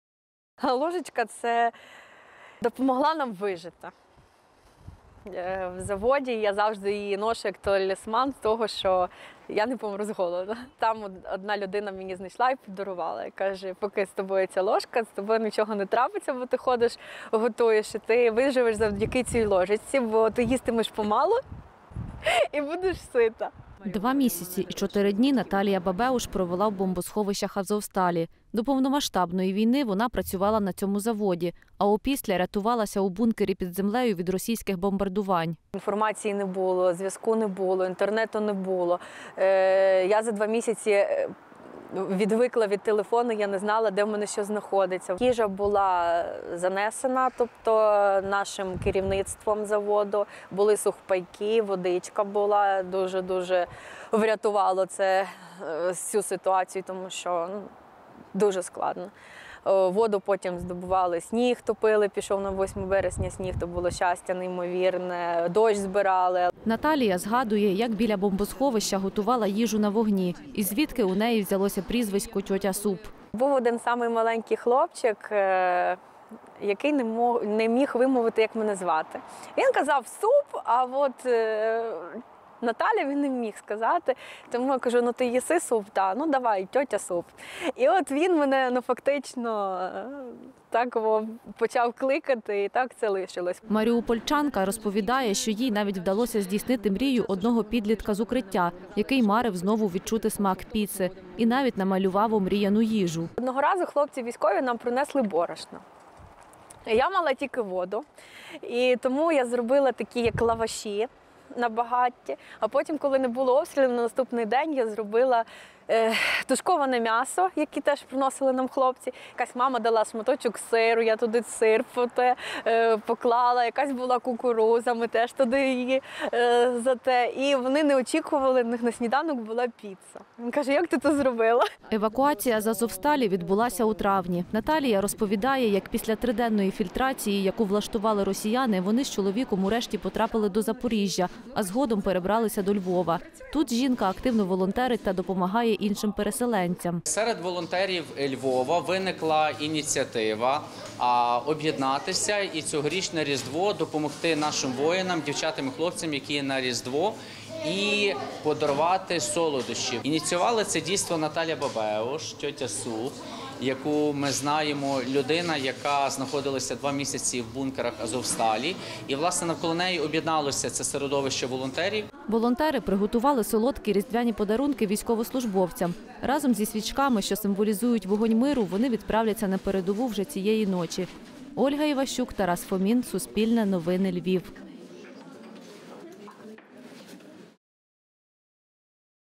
Як "Тьотя Суп" 2 місяці готувала їжу на "Азовсталі" та переїхала у Львів: історія переселенки
Мешканка Маріуполя Наталія Бабеуш понад два місяці провела в бомбосховищах заводу "Азовсталь". Там вона працювала до повномасштабного вторгнення. Знаючи про укриття на заводі, разом чоловіком та іншими цивільними вона ховалась від обстрілів росіян. Увесь цей час готувала їжу. Зараз вона живе у Львові та волонтерить. Більше про жінку на прізвисько "Тьотю Суп" дивіться у сюжеті.